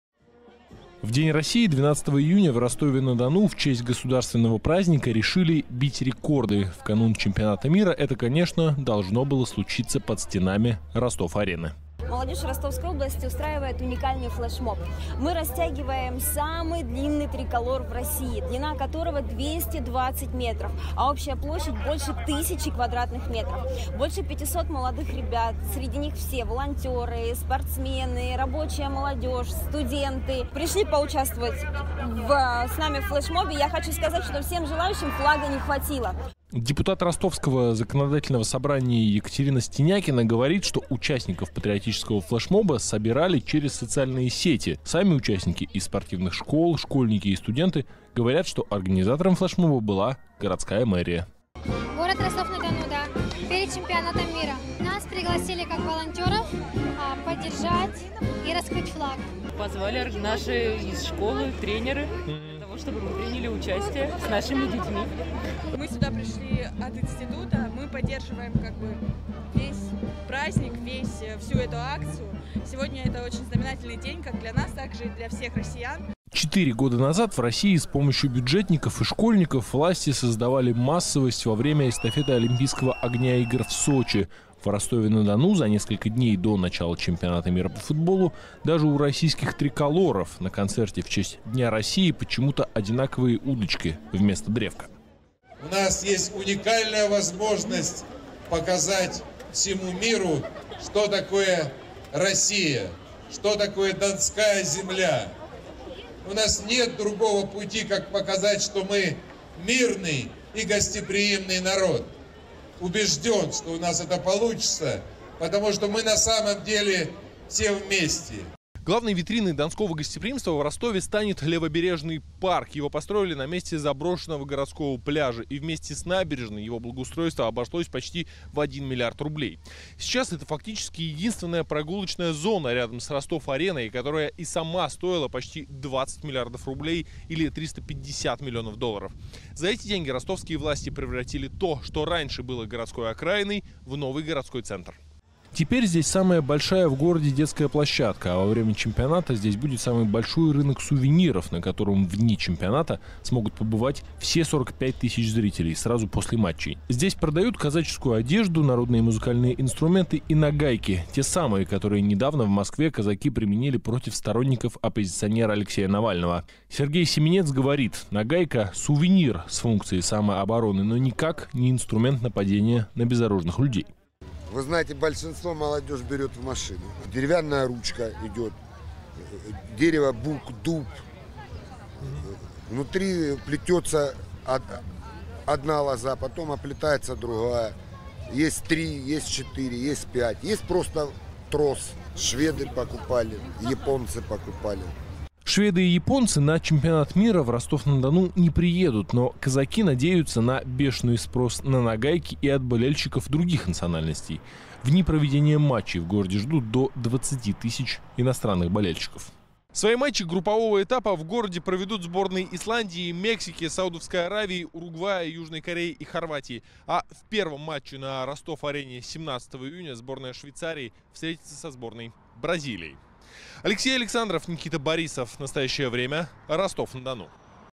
Как Ростов-на-Дону подготовился к ЧМ-2018
Самый длинный триколор, народные музыкальные инструменты и нагайки